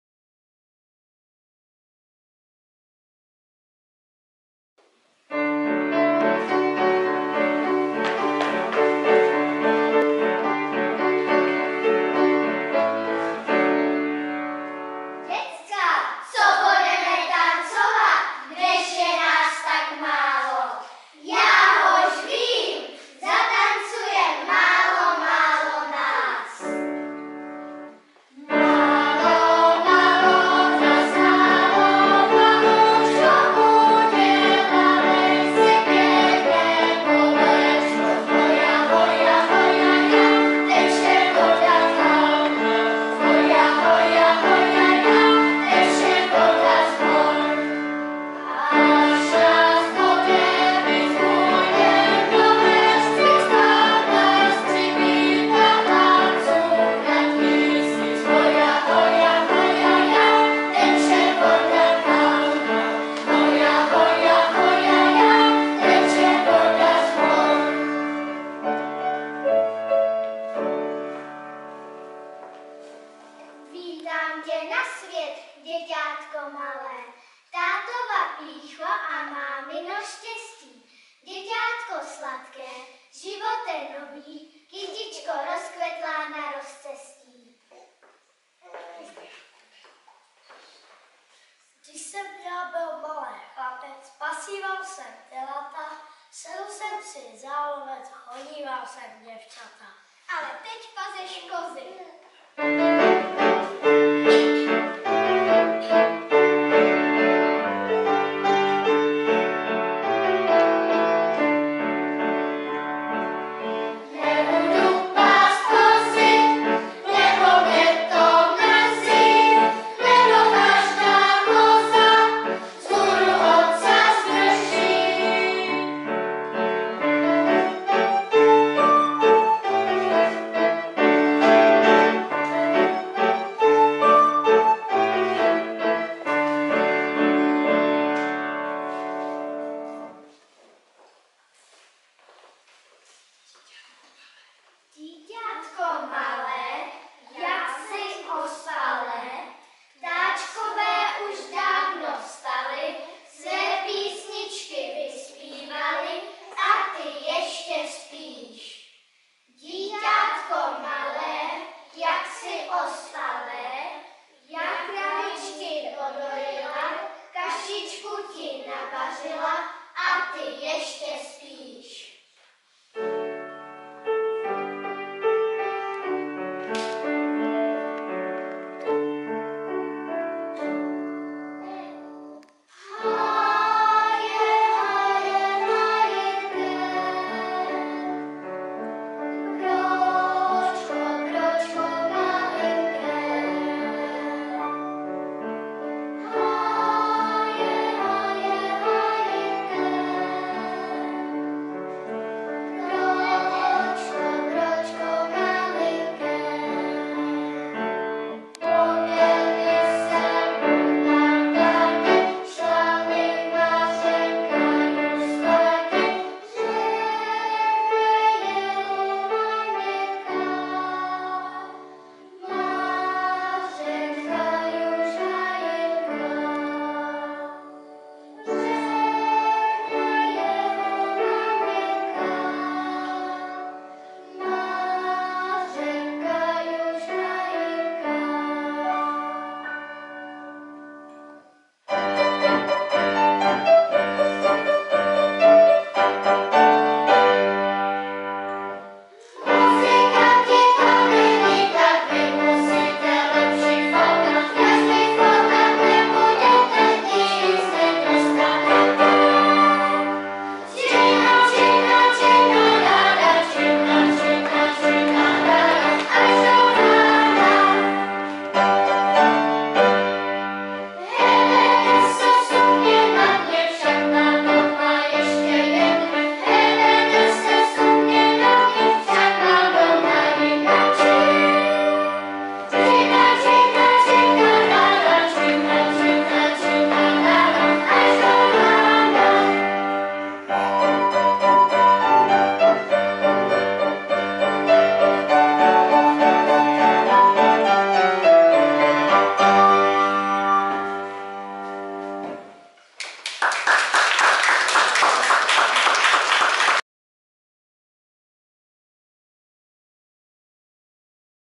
2022 - 12. listopadu - Komíňáček na podzimním Vítání občánků
Folklórní soubor Komíňáček zajistil kulturní náplň podzimního Vítání občánků v Komíně.

Hudební doprovod: Eva Burešová a Anna Veškrnová.
Vedení souboru: Ludmila Caletková a Eva Burešová.